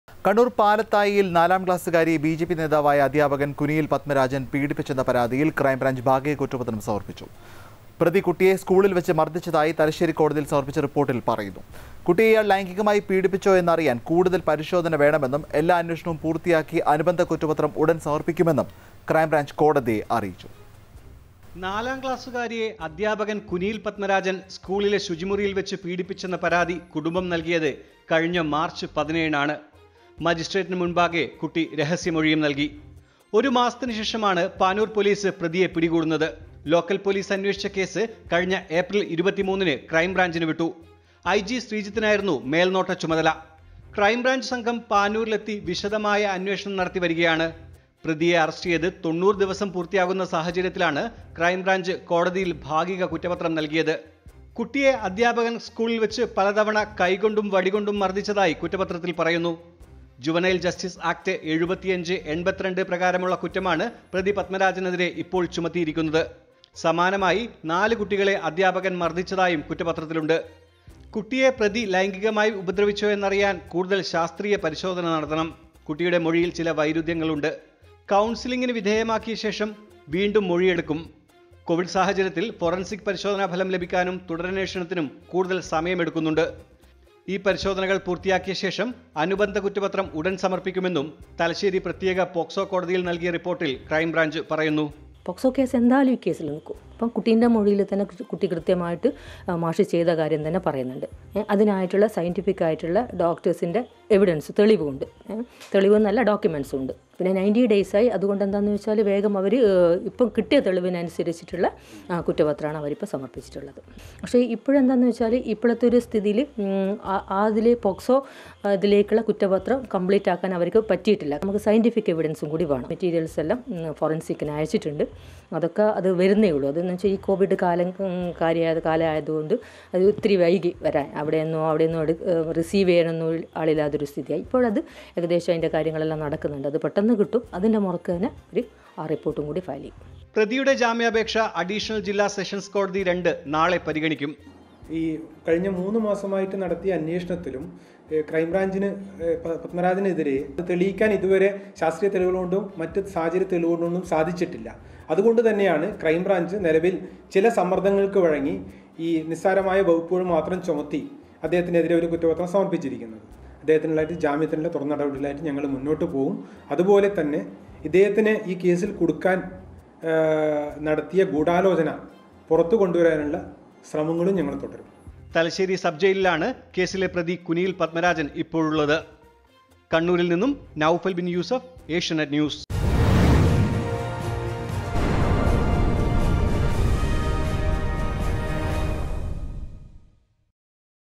പാലത്തായി പീഡനക്കേസ്: കുനിയിൽ പദ്മരാജനെതിരെ സമര്‍പ്പിച്ചത് ഭാഗിക കുറ്റപത്രം | palathayi rape case
Palathayi Child Abuse case police crime branch charge partial charge sheet